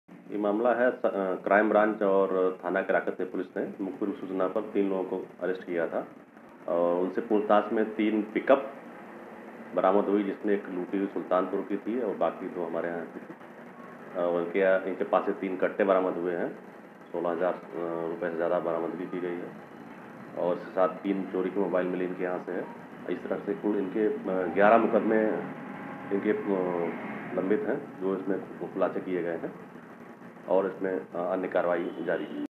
जौनपुर: अंतर्जनपदीय गिरोह का भंडाफोड़, इनामिया सहित तीन गिरफ्तार
जौनपुर: केराकत पुलिस और क्राइम ब्रांच की संयुक्त कार्रवाई में अंतर्जनपदीय गिरोह का भंडाफोड़ करते हुए तीन आरोपियों को गिरफ्तार किया गया. इनके पास से चोरी की तीन पिकअप गाड़ी, 16 हजार नगद और तीन कट्टे के साथ अन्य उपकरण बरामद किये गऐ. आरोपियों के ऊपर विभिन्न थानों में 11 मुकदमें दर्ज हैं.